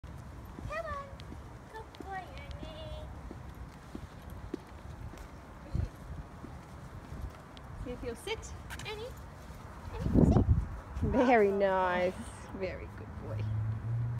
16 week old Bernese Mountain Dog Puppy walking on loose lead.
Want to know how to teach your puppy how to walk on the lead?
Want to get your kids involved in the process?

Get in touch with us now, Melbourne - Australia